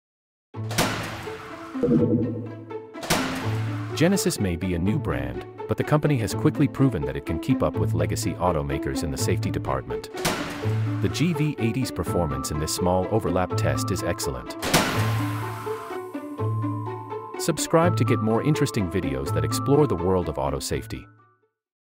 Genesis GV80 Crash Test
The Genesis GV80 scored top marks in the IIHS' small-overlap crash test. There was minimal intrusion into the occupant compartment and dummy movement was well controlled. Raw video footage courtesy of the IIHS.